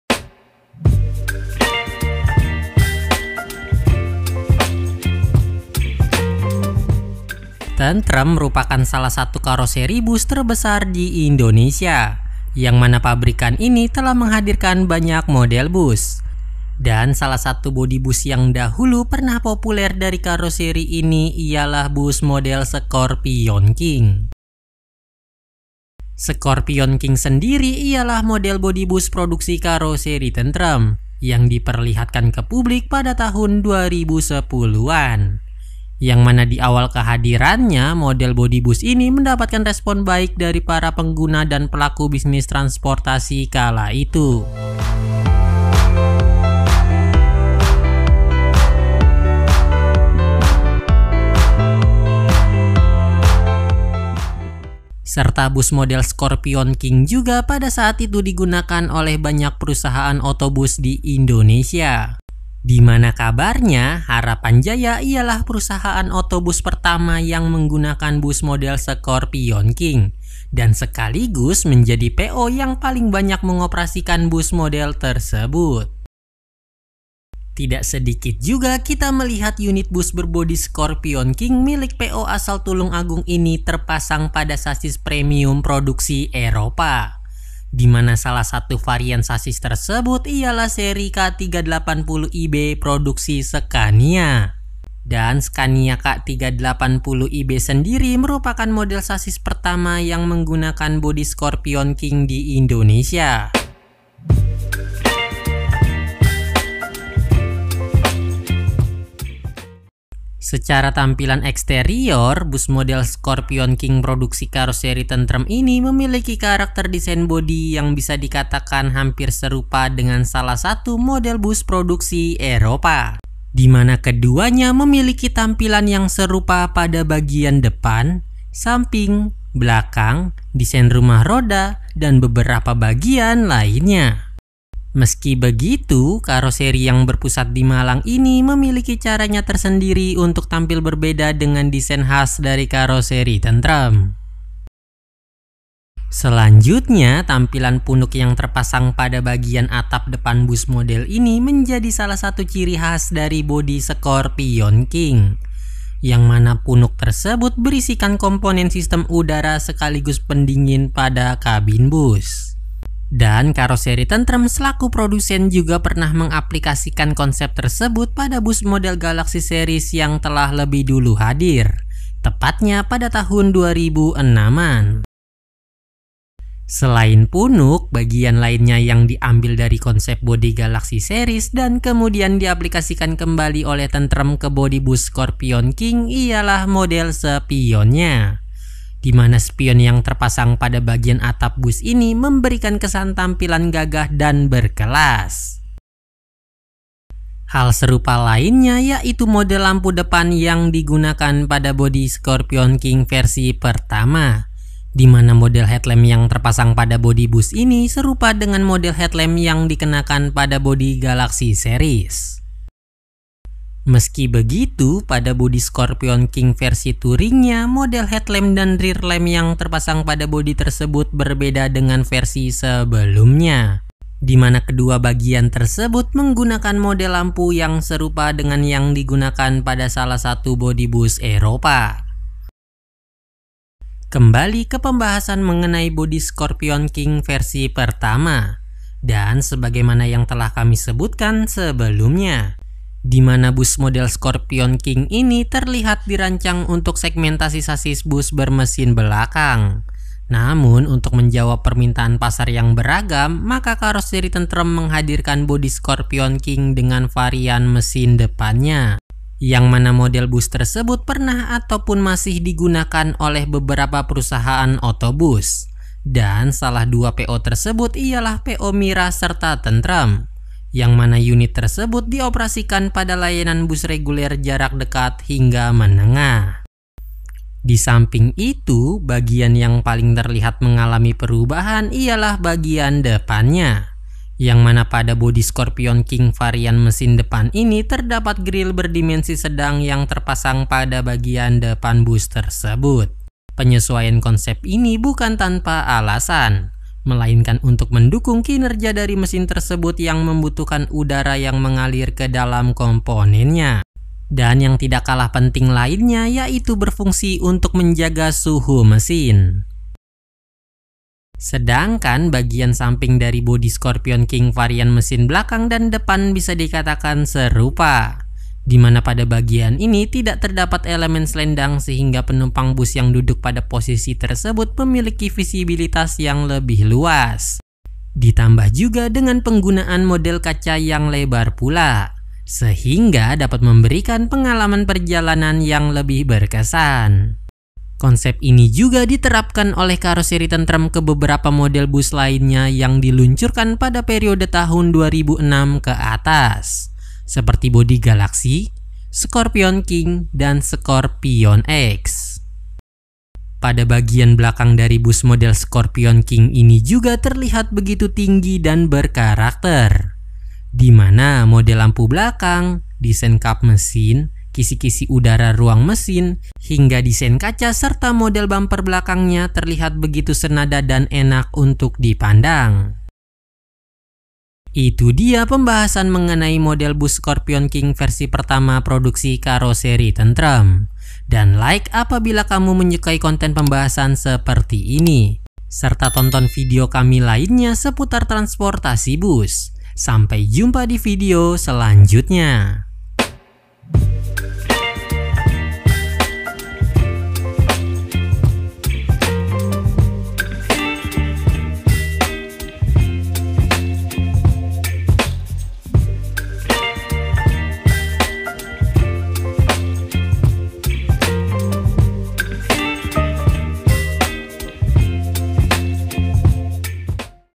MENGENAL BODY BUS SCORPION KING KAROSERI TENTREM
Halo NetBus, Kali ini mari kita mengenal body bus scorpion king karoseri tentrem.